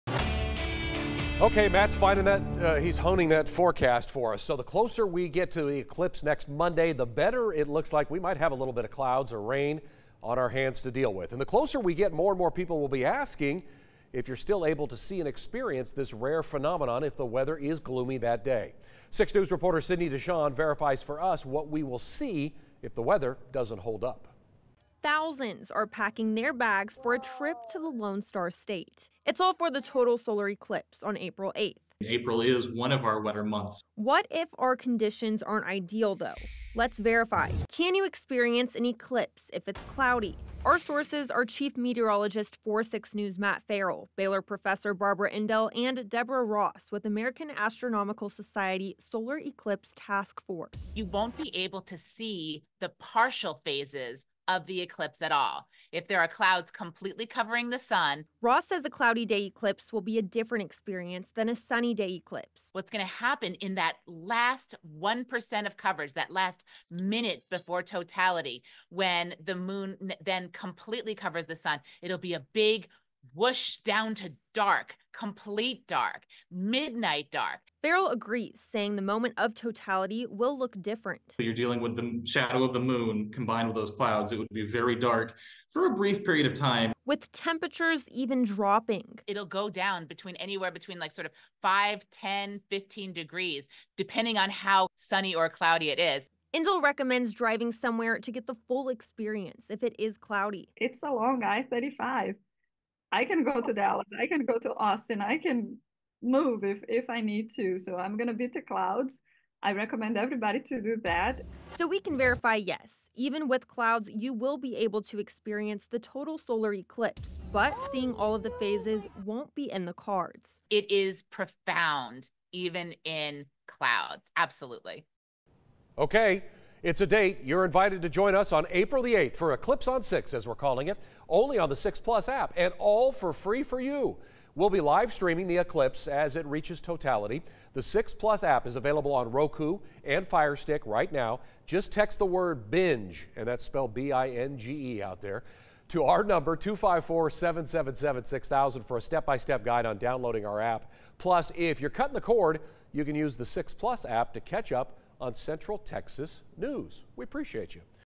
Verify | Can you experience an eclipse if it's cloudy?
We verify if you can still see an eclipse if it's cloudy.